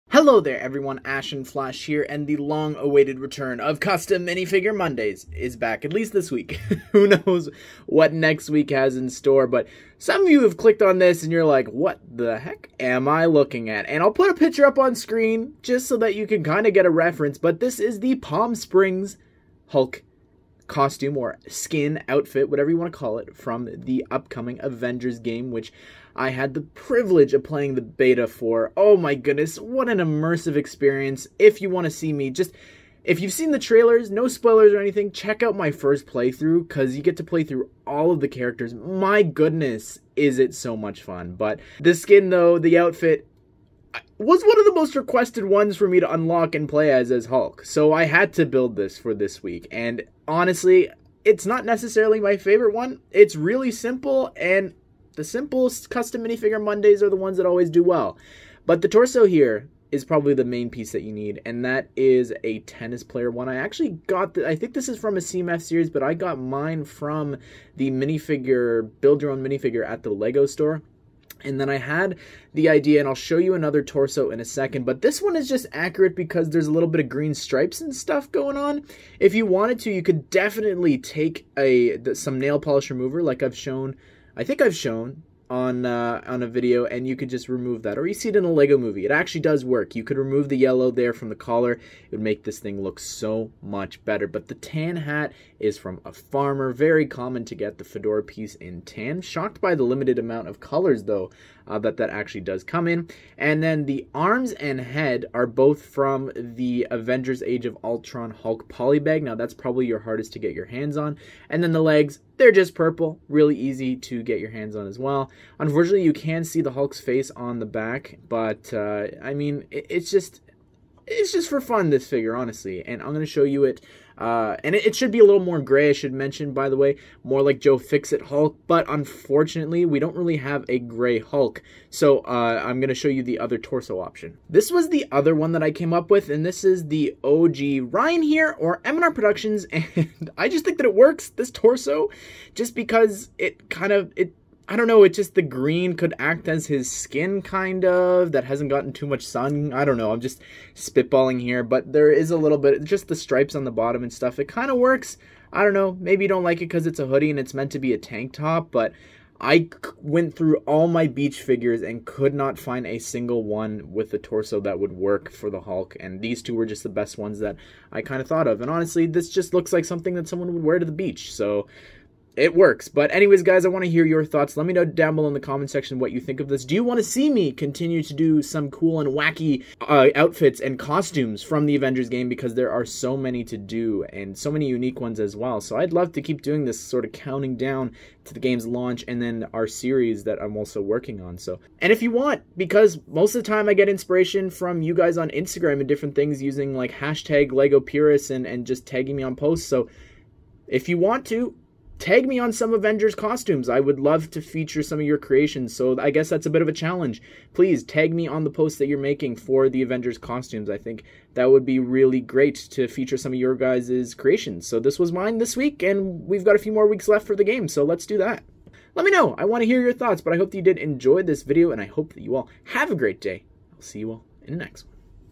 How to Make LEGO Palm Springs Hulk from the Avengers Game
Today's custom figure is of the new Hulk outfit from the upcoming Avengers Game! This is the Palm Springs Outfit!